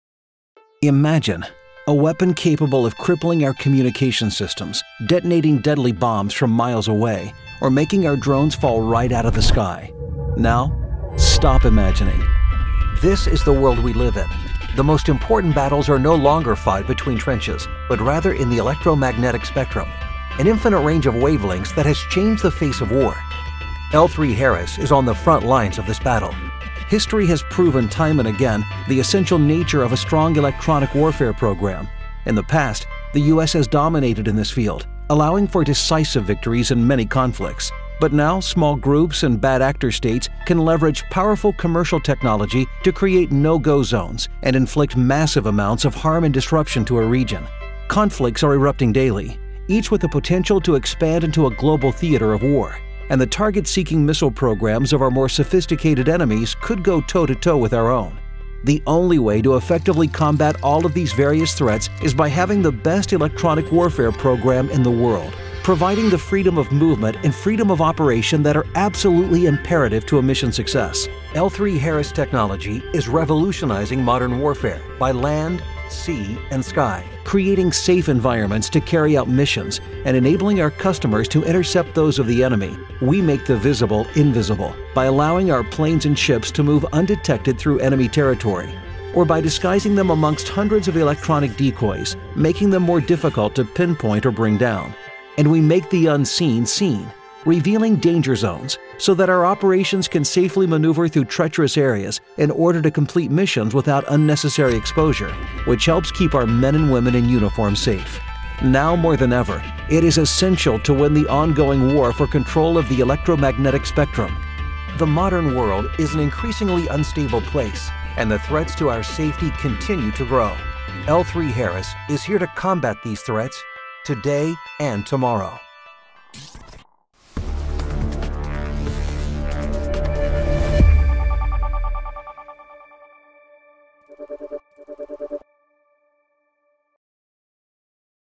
Revolutionizing Modern Electronic Warfare | L3Harris
L3Harris is developing products and capabilities to help the warfighter adapt to an ever-changing environment.